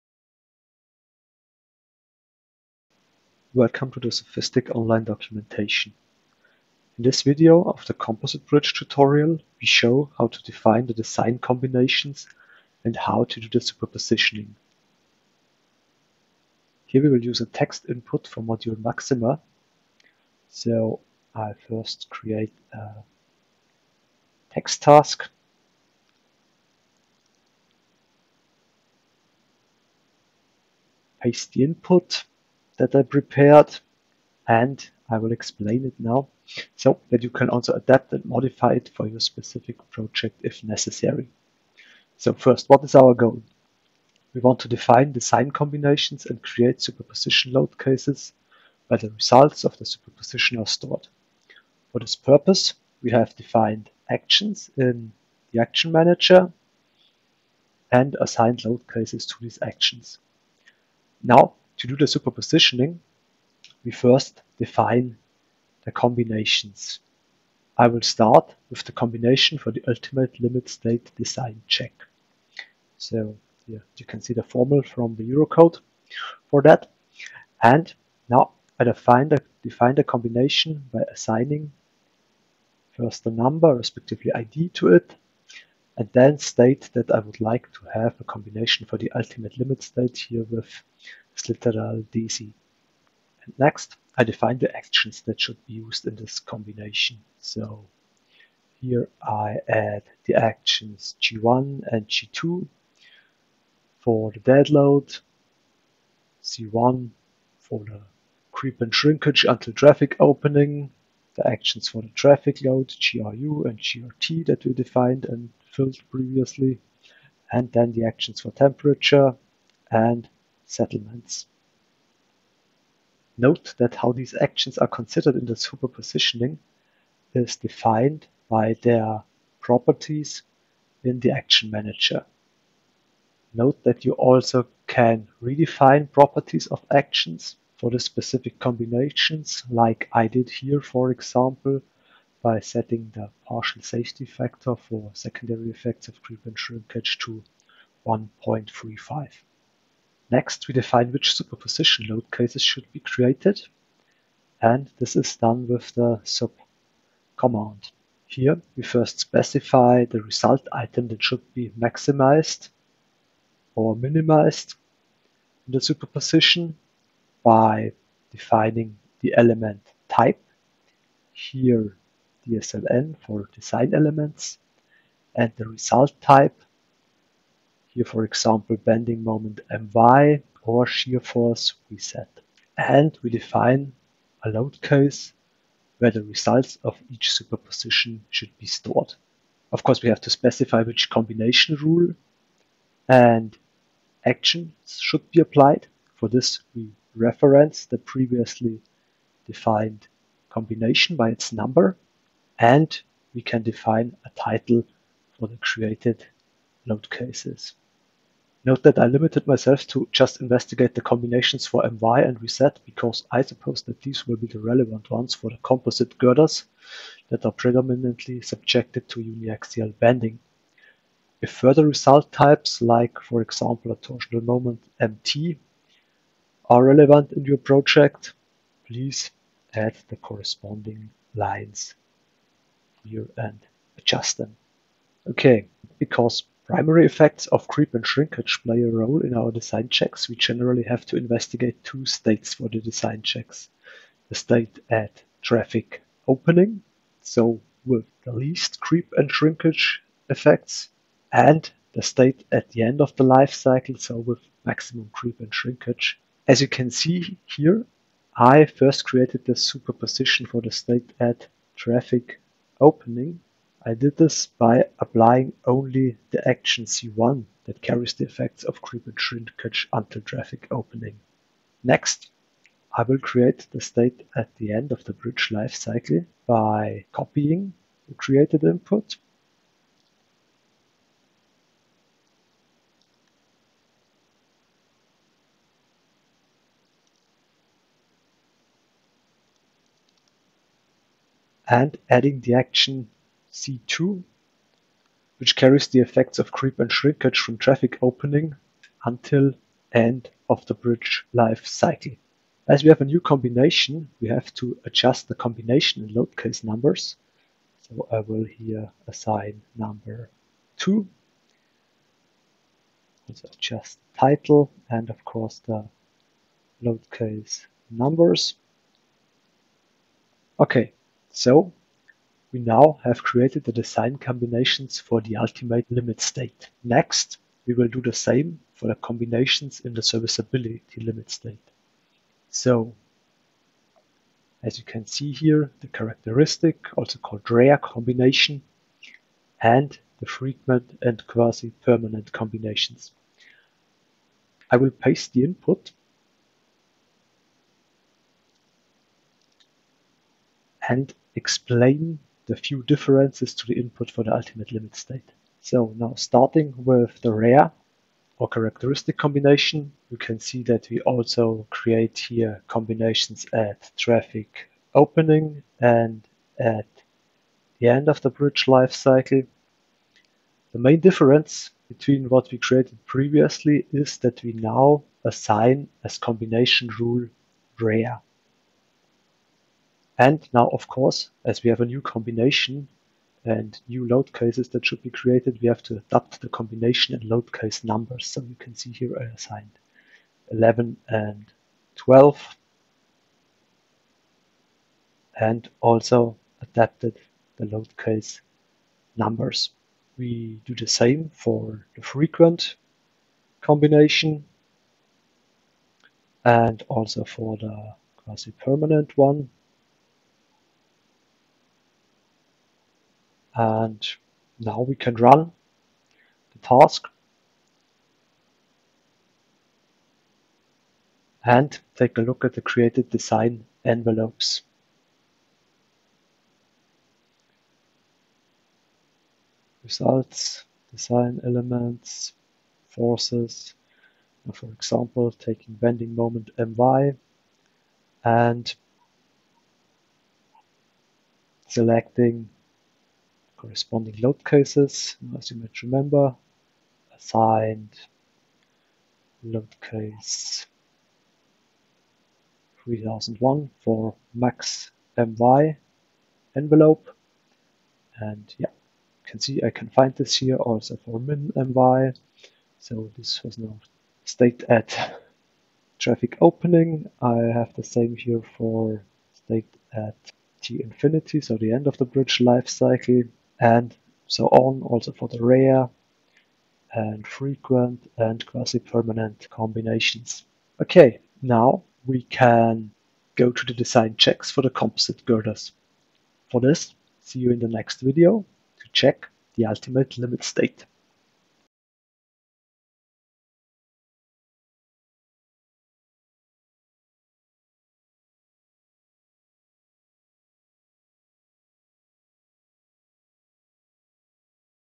Composite Bridges: Design Combinations
This video shows how to define the design combinations and how to do the superpositioning.